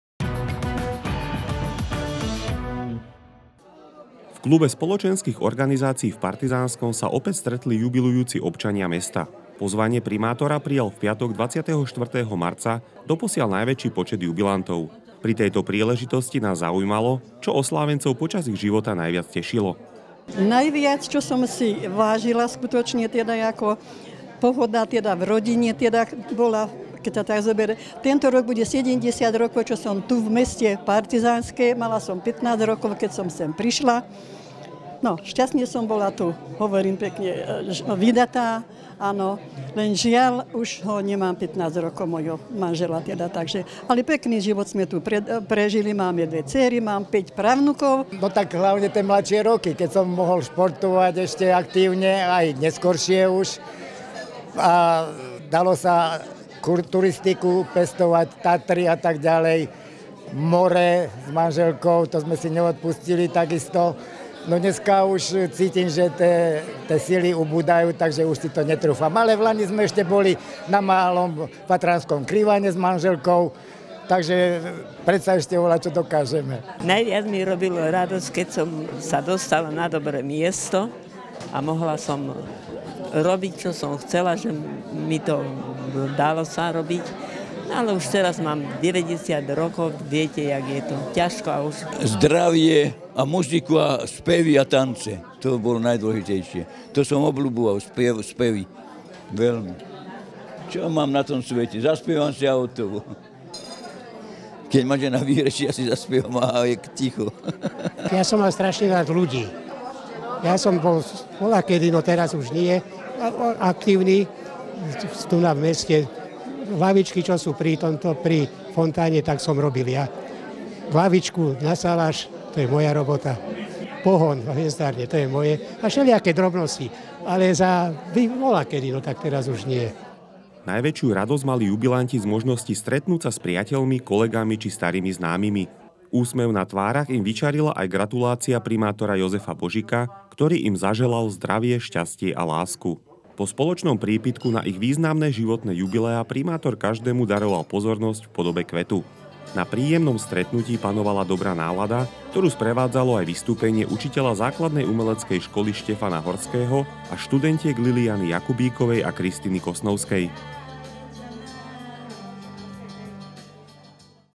Oslávili životné jubileum
Pozvanie primátora prijal v piatok 24. marca doposiaľ najväčší počet jubilantov.